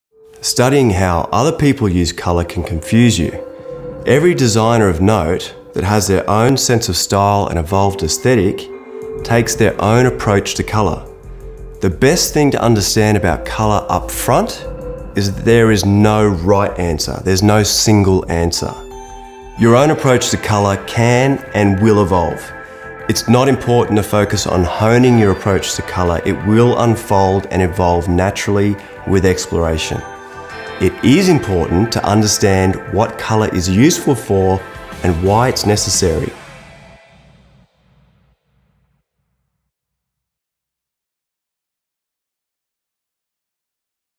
Colour: Module 6 from MASTERSERIES
Colour is featured in MASTERSERIES, an Interior Design informative masterclass curated by Darren Palmer with the team from The Interior Design Institute. MASTERSERIES contains 15 videos in total: 12 modules and three-part Q&A featuring Darren Palmer, and a downloadable eBook to complement the content covered in the Interior Design course.